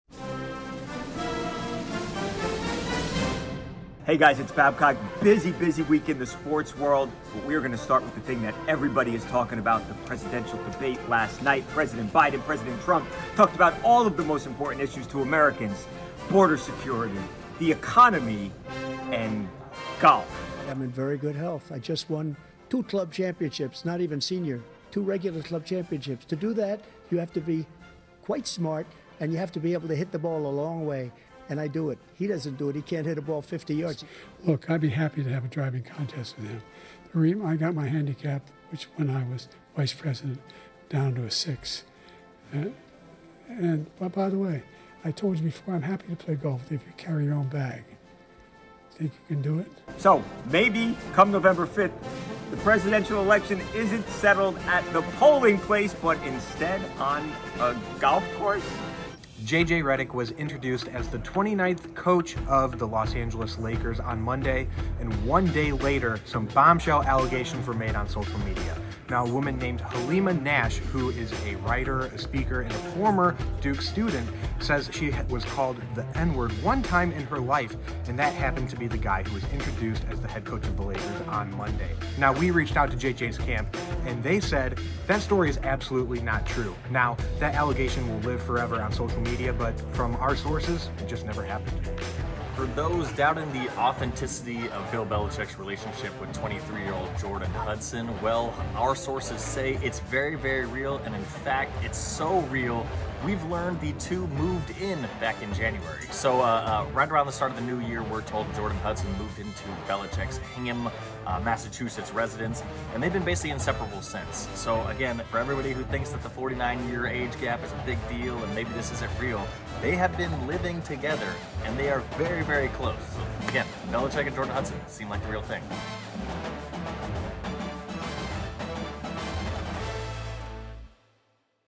In Case You Missed It, June 28, 2024 | TMZ Sports
Here are some of the top stories of the week, "In case you missed it". The Presidential debate was a hot topic after Thursday night and based on the best argument of the night, it looks like the 2024 election could get settled on the golf course. Plus, newly appointed Lakers head Coach JJ Redick is facing accusations he used a racial slur directed towards a campus volunteer while at Duke. And, Bill Belichick has always taken everything on the football field serious. His new relationship looks to be no different.